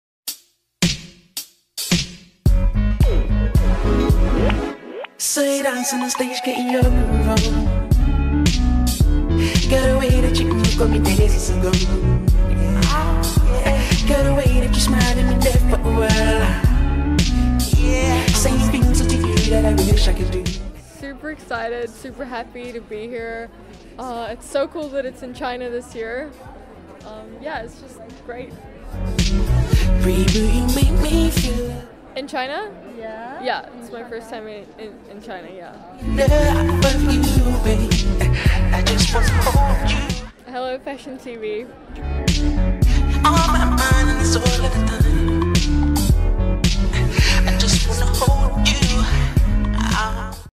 Victoria's Secret Fashion Show 2017 Shanghai Backstage ft.Kelly Gale Part.7 | FashionTV
SHANGHAI - FashionTV brings you more exclusive interviews from the Victoria's Secret Fashion Show 2017 backstage! Find out what model Kelly Gale thinks about the show this year !